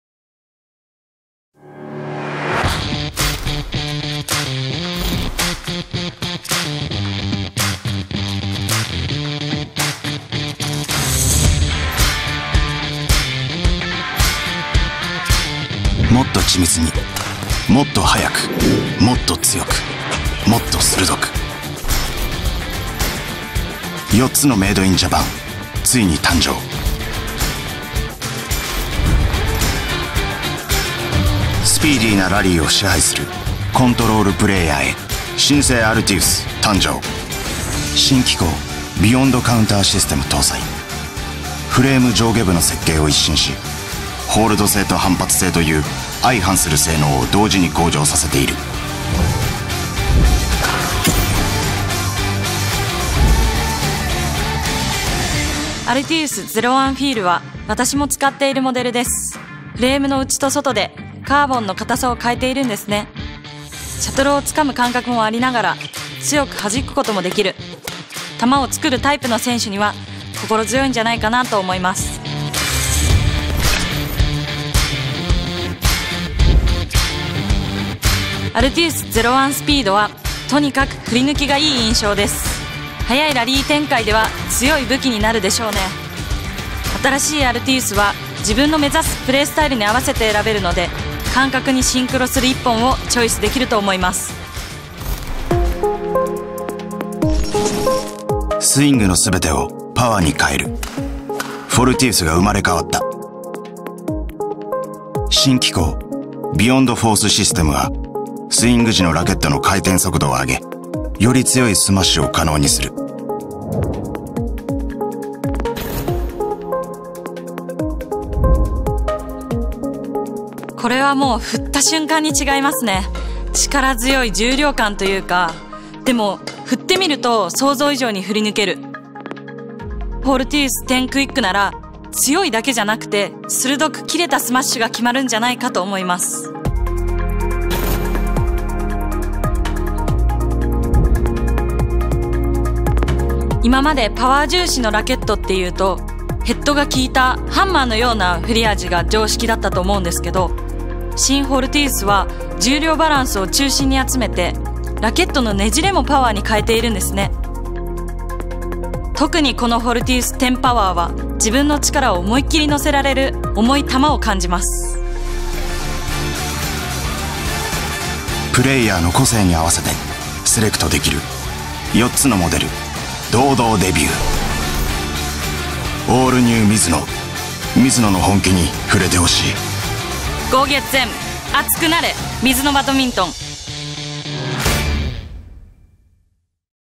MIZUNO BADMINTON-FEEL、SPEED、QUICK、POWER四款球拍介紹
【全新四款日製拍 滿足最挑剔的你】
ALTIUS / FORTIUS 系列為你獻上，
FEEL、SPEED、QUICK、POWER四款球拍，
順應你的打法，找出最適合的球拍吧‼
這次特別邀請MIZUNO羽球大使 奧原希望，